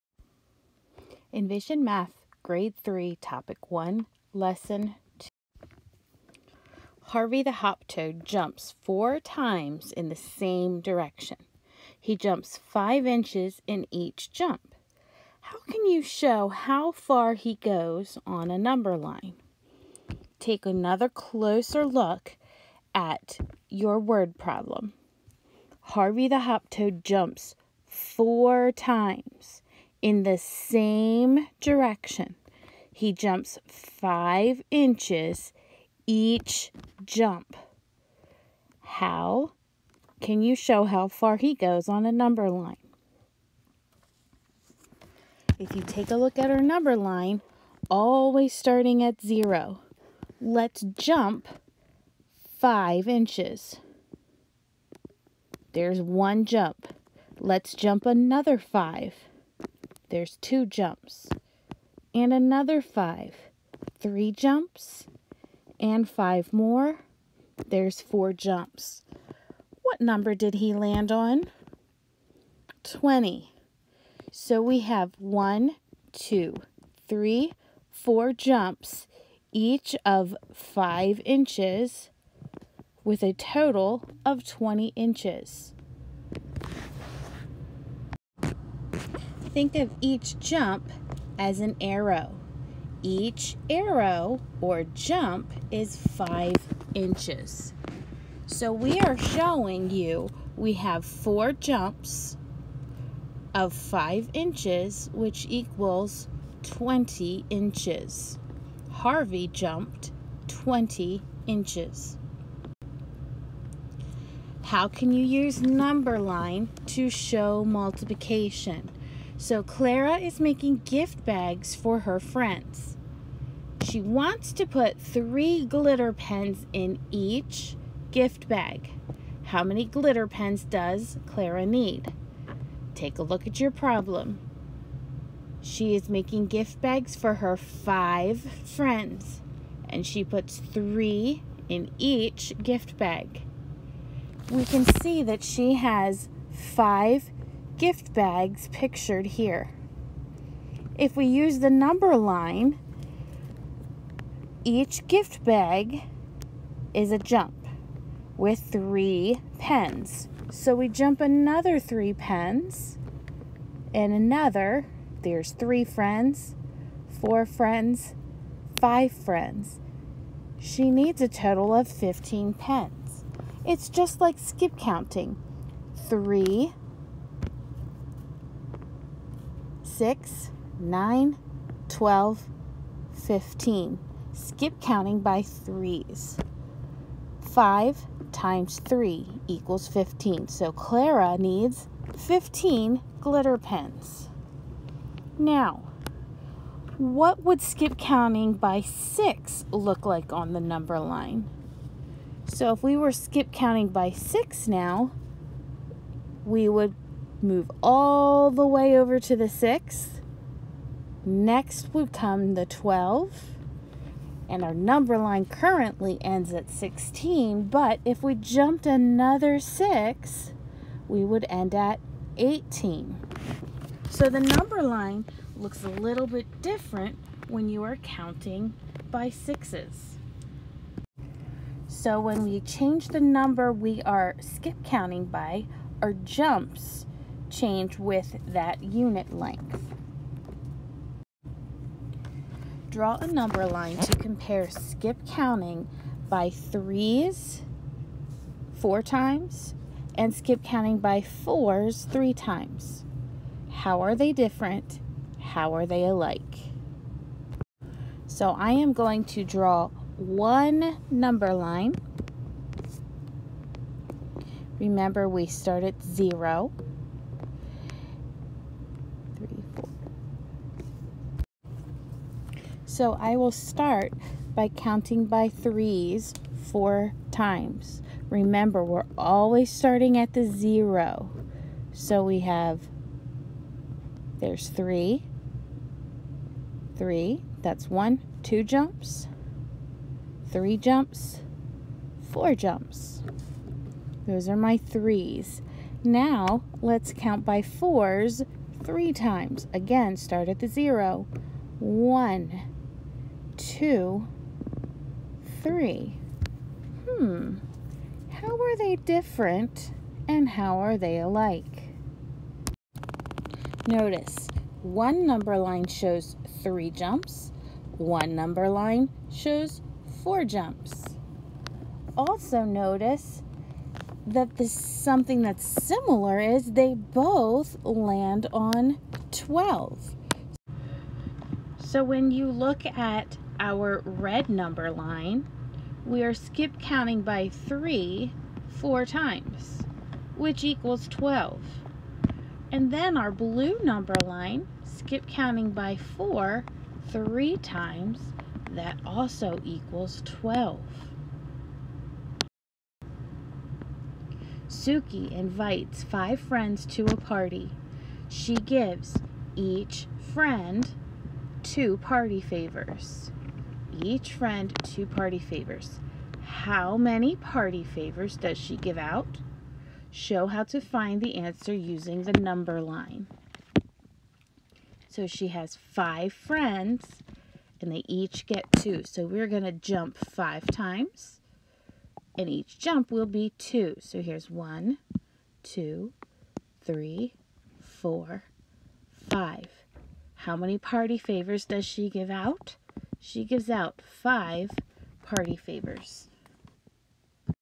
Envision Math 2.0 Grade 3 Topic 1 Lesson 2, 1-2, Multiplication on a number line
I use these videos to front load and flip my classroom. This video is used with an Edpuzzle the day before each lesson for students to preview and practice. It is also a good resource for parents to see how the lesson was taught and to be able to help their child with homework.